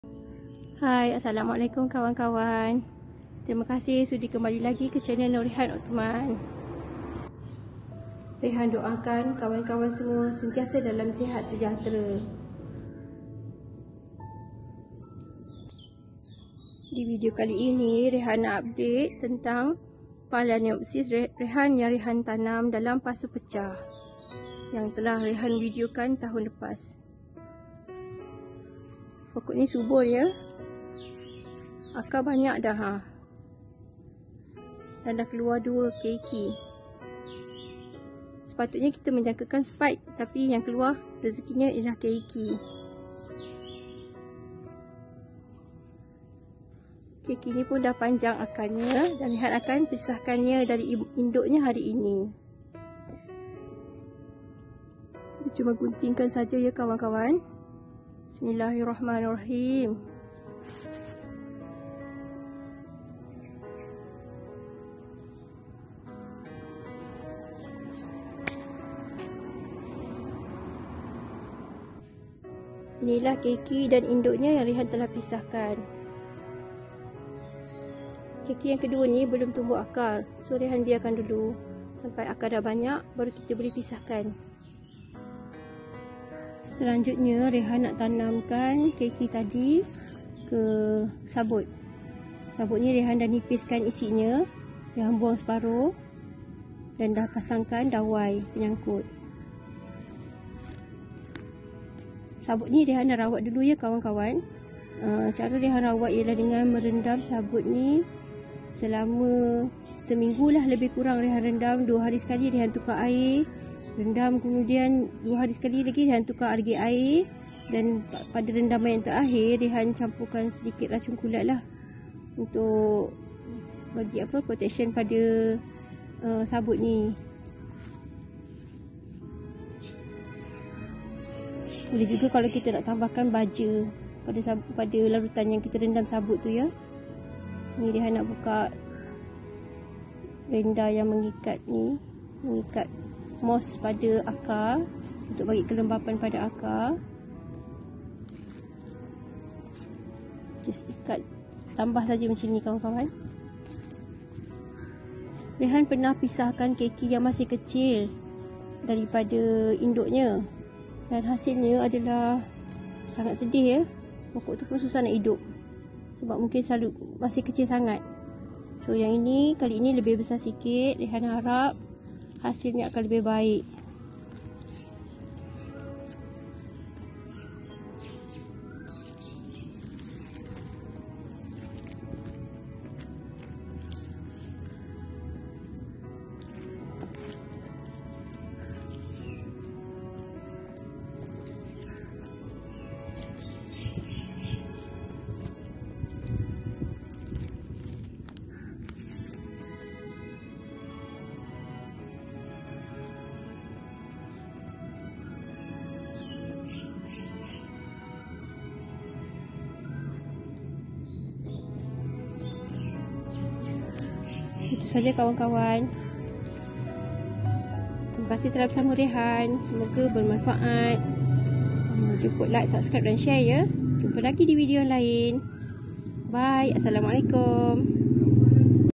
UPDATE : Phal dipasu pecah & Tanam keiki disabut. | Norehan Othman
As Salam kawan kawan.. Pada video kali ini Rehan update tentang orkid Phalaenopsis yang Rehan tanam dalam pasu pecah. Juga cara Rehan lekatkan keiki Phal pada sabut kelapa.
Cara Rehan rawat sabut sebelum digunakan:
1. Rendam sabut dalam air selama 2 hari.
2. Ganti air yang baru, rendam lagi untuk 2 hari.
3. Ganti air yang baru, tambah racun kulat/baja B1, rendam lagi untuk 2 hari.
4. Keringkan air dan jemurkan sabut.
5. Bolehlah dicincang kecil atau gunakan terus sabut.